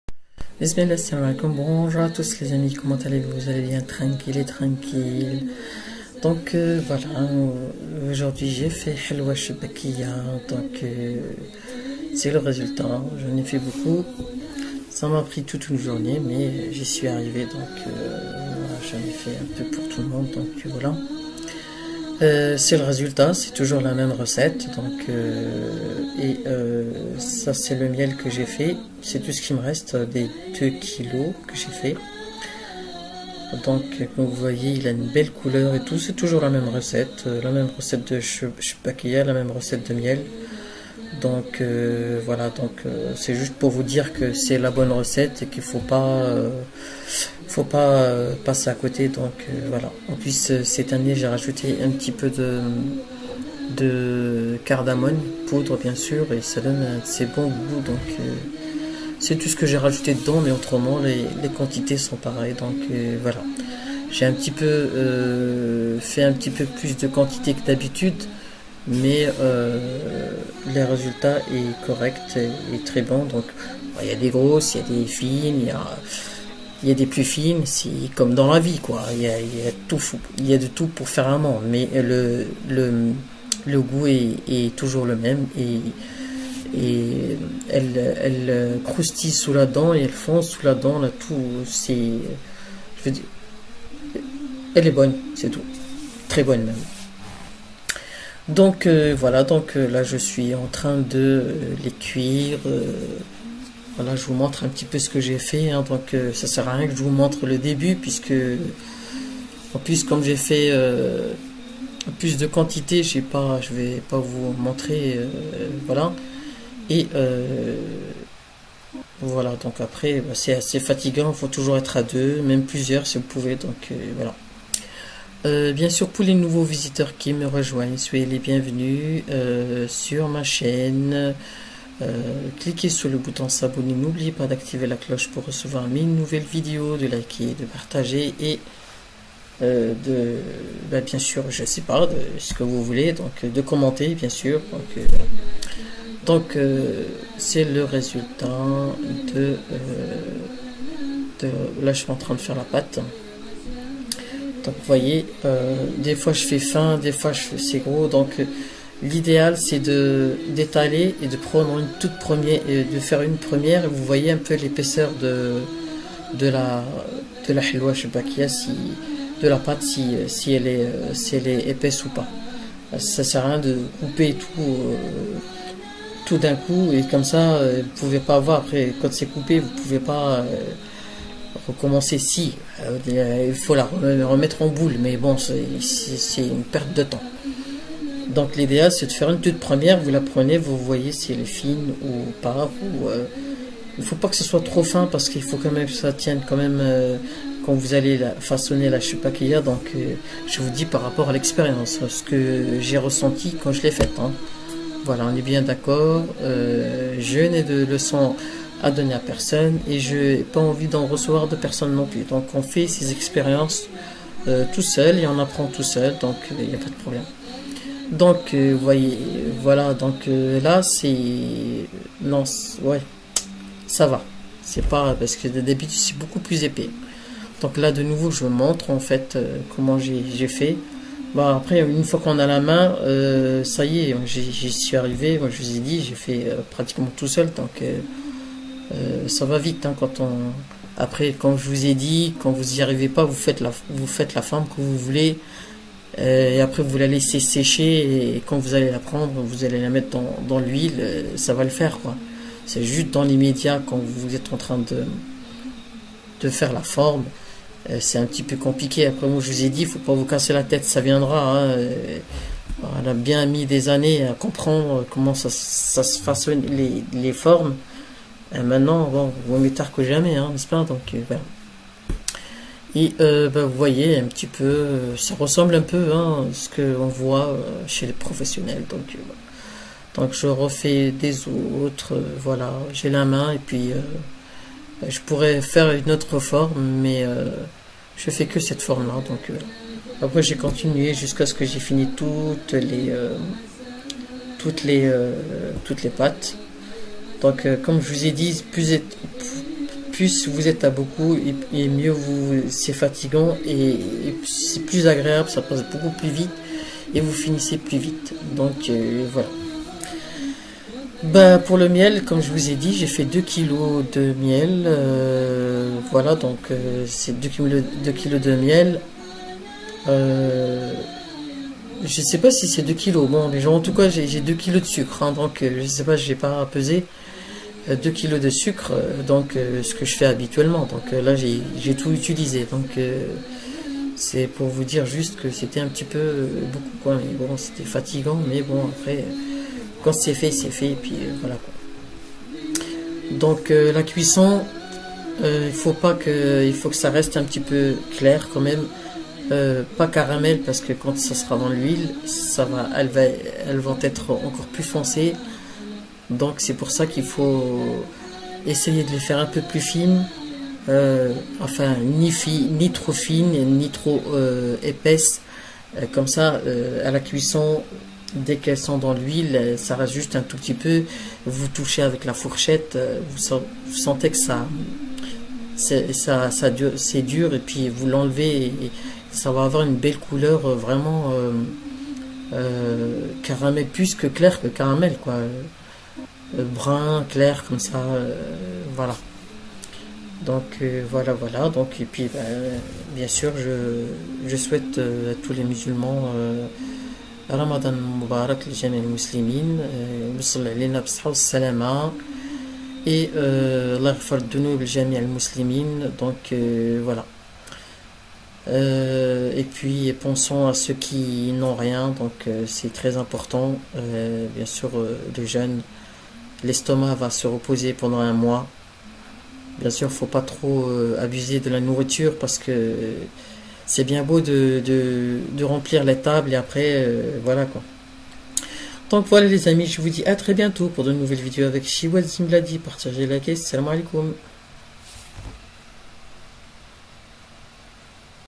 Chebakia Suite cuisson 2022 ! un délice a voir r !/حلوة شبكية سهلة/halwa chebakia easy
طريقة سهلة و سريعة لطي او تشبيك الشباكية , المخرقة , chebakia marocaine facile
رمضان سعيد لجميع المسلمين!
Bon Ramadan a tous les
musulmans
 Halwa chbakia ! toujours la même recette et toujours le même résultat et le même gout !
N'OUBLIEZ PAS LES LIKES POUR ME SOUTENIR ET PARTAGER !